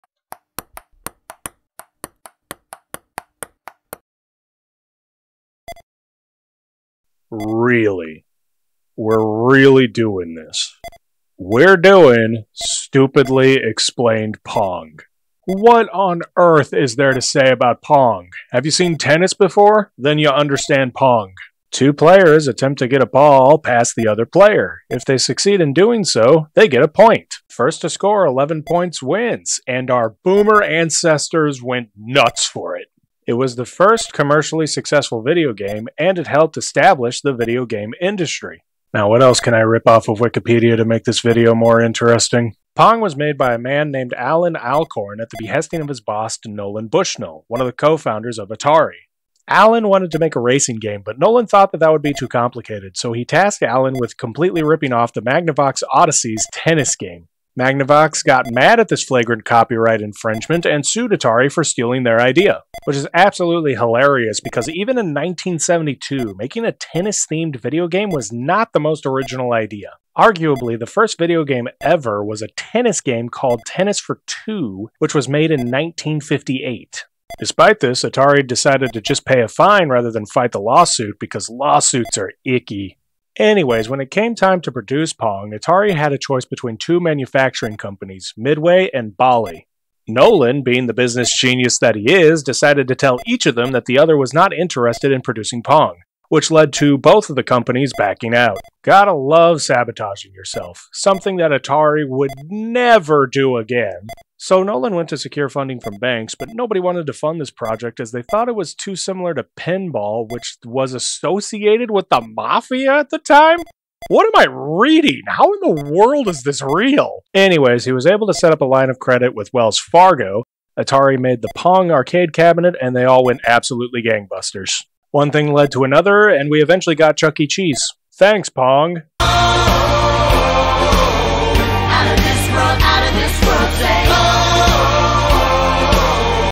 Stupidly Explained: Pong
Let's explore the history of Pong as dumbly as possible.